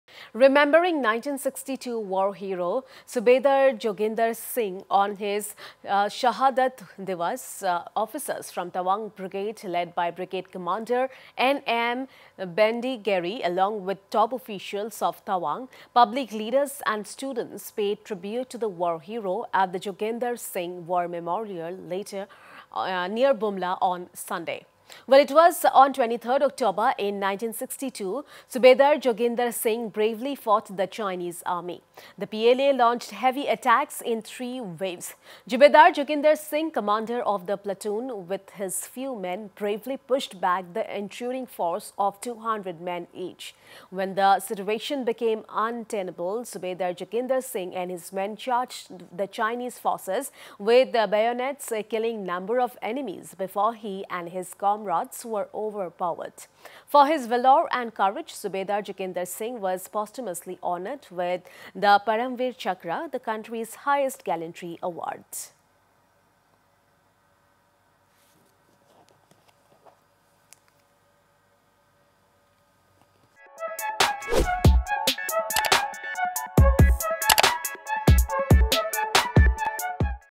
Tawang pays tribute to 1962 war hero Joginder singh
Remembering 1962 war hero Subehdar Joginder Singh on his Shahadat Diwas, officers from Tawang brigade led by Brigade Commander N M Bendigeri, alongwith with top officials of Tawang, Public leaders and students paid tribute to the war hero, at the Joginder Singh war memorial near Bumla on Sunday.